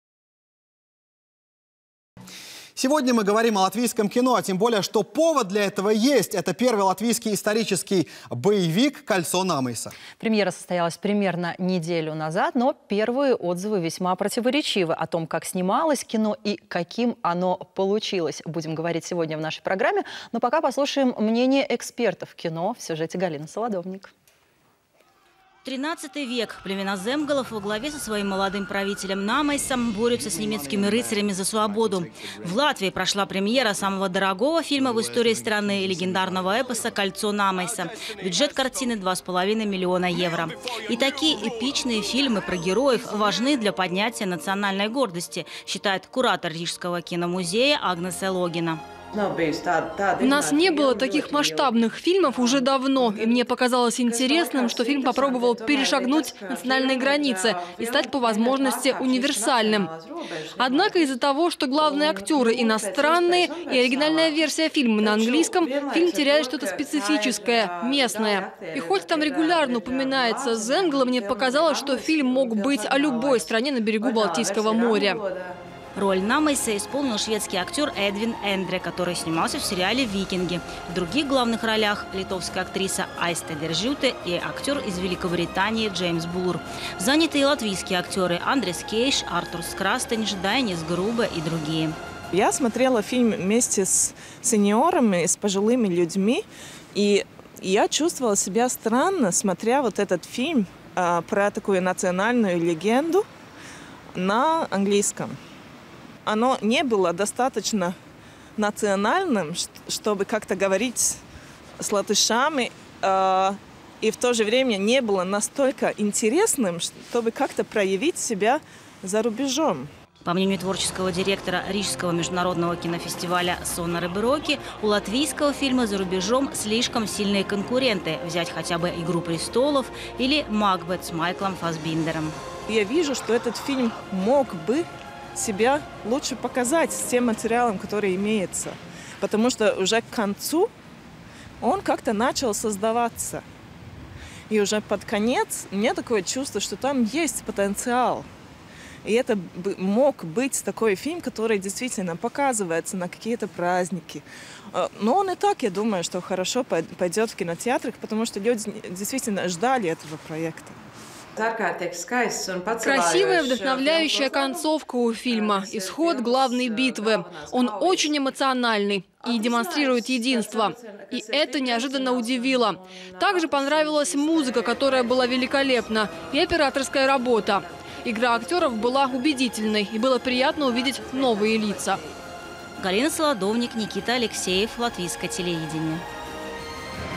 Кольцо Намея
Сегодня мы говорим о латвийском кино, тем более, что повод для этого – первый латвийский исторический боевик «Кольцо Намейса»
Премьера этого фильма состоялась примерно неделю назад и отзывы о фильме весьа противоречивые. О том как снималось кино и каким оно получилось – говорим с создателями фильма, но перед этим мнение профессионалов  в сюжете Галины Солодовник